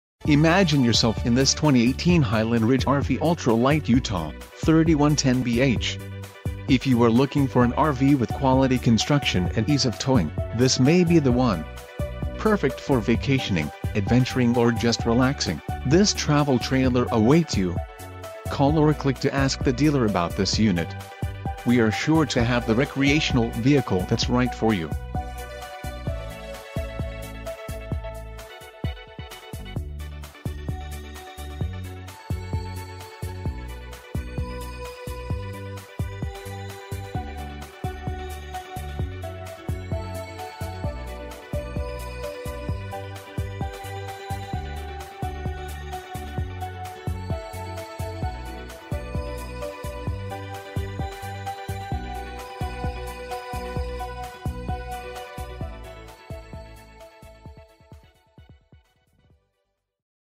2018 Highland Ridge RV Ultra lite UT3110BH - Edwards Mot...
2018 Highland Ridge RV Ultra Lite UT3110BH SUV's Love The Ultra Lite Highland Ridge has expanded into a new market segment with the introduction of the Open Range Ultra Lite! The product consists of two models (rear living &amp; bunk model) and will continue to grow steadily to include 12 floor plans by June 2017. With an approximate weight of 5,500 pounds, most medium-duty trucks and SUVs can tow the new Open Range Ultra Lite. In keeping with Open Range tradition, the new Open Range Ultra Lite has been designed from the ground up with the concept of "more for less". Whether it is the 2-year warranty, 26" full width entry door (2), full walk-in 5'10" slide height, the full depth 39" slide with 42" dinette, or the vinyl flooring throughout the entire unit.